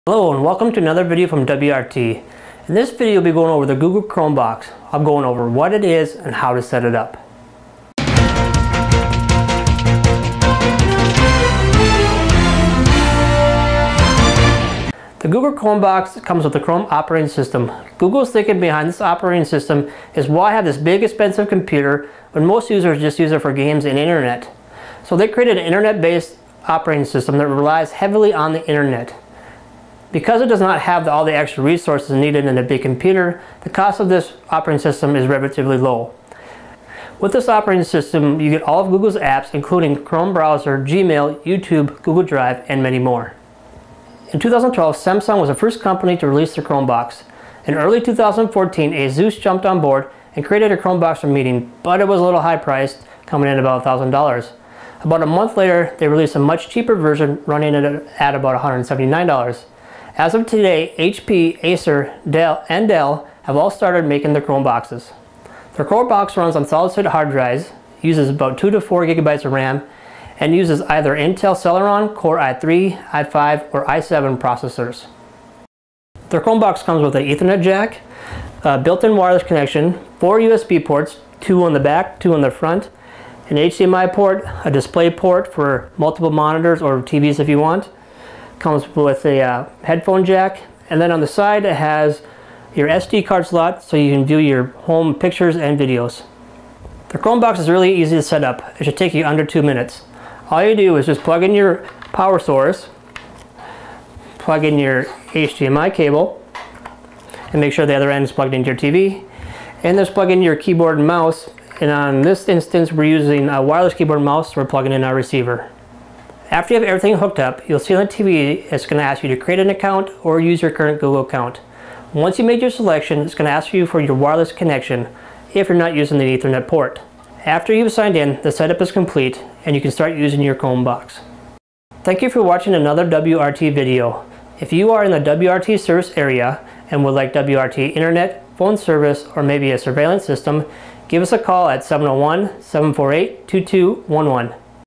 What is a Chromebox and how to set it up?
Learn what a chromebox is plus the thinking behind the Chrome Operating System. We will also show you how to hookup your chromebox.